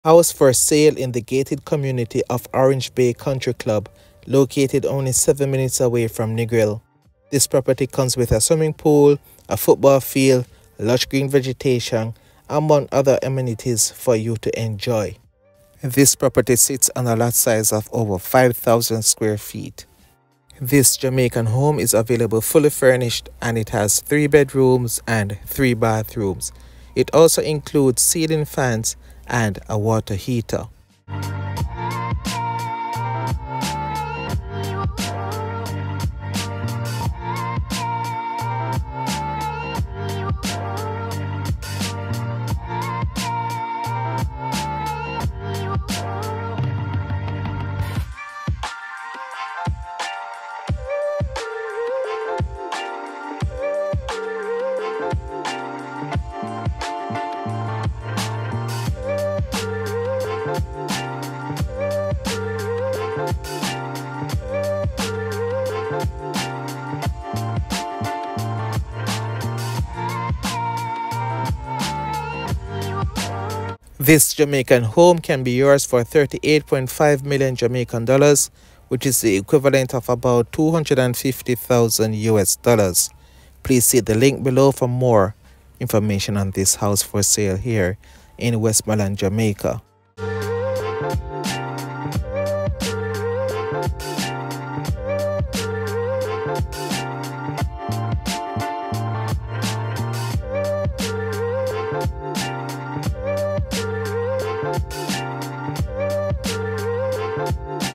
GATED FURNISHED HOME FOR SALE NEGRIL🇯🇲
House for sale in the gated community of Orange Bay Country Club located only 7 minutes away from Negril Westmoreland Jamaica. This Jamaican home is available fully furnished and offers 24 hours security, swimming pool and a football field for your enjoyment.